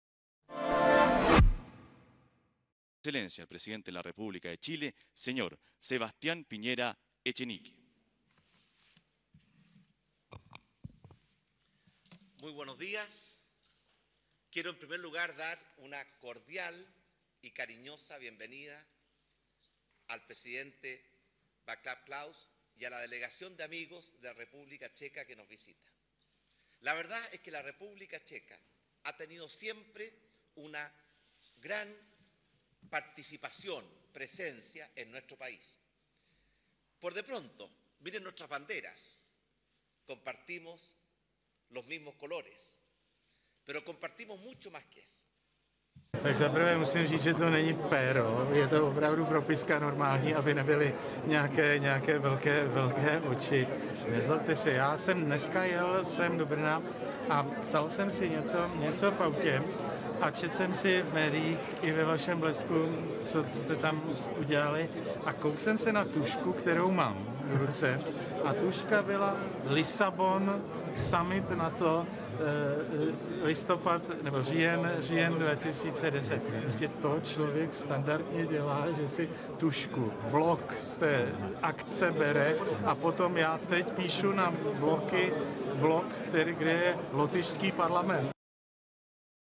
Czech president steals pen
Czech President Vaclav Klaus has caused a diplomatic stir after a video showed
  him discreetly pocketing a ceremonial pen during a visit to Chile.